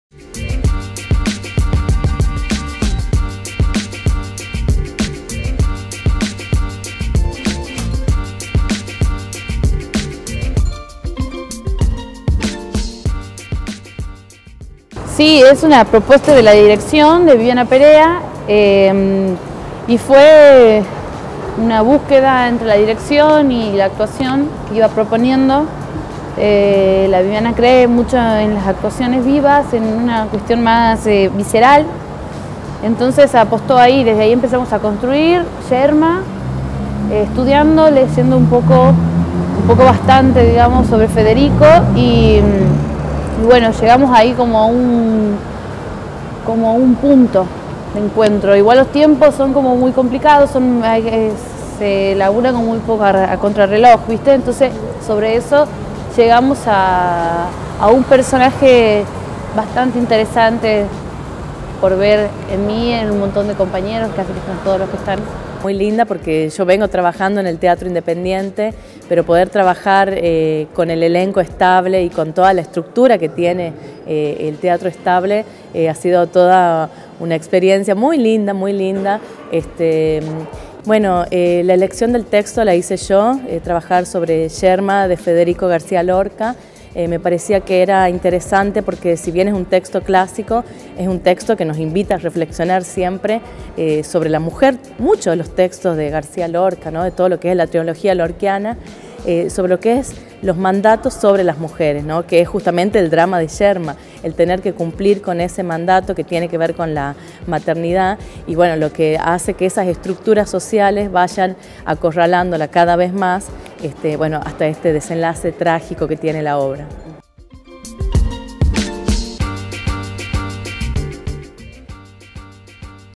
El Teatro Estable presenta “Yerma” a la sala Orestes Caviglia
Protagonizada por actores del Teatro Estable de la Provincia, “Yerma” sube a escena en la Sala Orestes Caviglia (San Martín 251), el próximo sábado 16 de junio, a las 22 horas, después de su gira en el marco del Ciclo El Teatro Estable en el Interior” que comenzó en mayo. Las próximas funciones serán, el 22, 23, 29 y 30 de junio.
Situada en la actualidad, esta nueva propuesta que dirige Viviana Perea, se centra en una mujer atormentada por los mandatos sociales y la necesidad de tener un hijo.  El grito por el deseo de vida y libertad revoluciona y alborota su cuerpo y se manifiesta en cada una de sus acciones hasta llevarla a la locura.
La obra se enfoca sobre las relaciones en torno a lo familiar, la amistad, el deseo y la sociedad. La  necesidad de tener un hijo lleva a Yerma a un camino sin salida donde se encuentra  presa de sus deseos y de una estructura social que no da respiro. A diferencia de la Yerma del campo, se trata de una propuesta contemporánea, pero se mantiene el texto y la poesía de Lorca.
El elenco está integrado por Ruth Plaate en quien recae el protagónico (Yerma); Joel Alonso interpreta el personaje de Víctor e Ignacio Hael es Dolores, la conjuradora; el elenco se completa con Mariano Juri (Juan), Eloísa Martínez Romero (María), Micaela Rojas (Muchacha); Andrea Barbá, Daniela Villalba, Daniela Canseco y Alejandra Páez Salas (Mujeres).

FICHA TÉCNICA
Puesta en escena y dirección actoral: Viviana Perea
Asistencia de producción: Silvina Schliserman
Asistencia de dirección: Marcela García Mottes
Coordinación técnica: Julio Rojas
Diseño de Escenografía: Sofía Seidán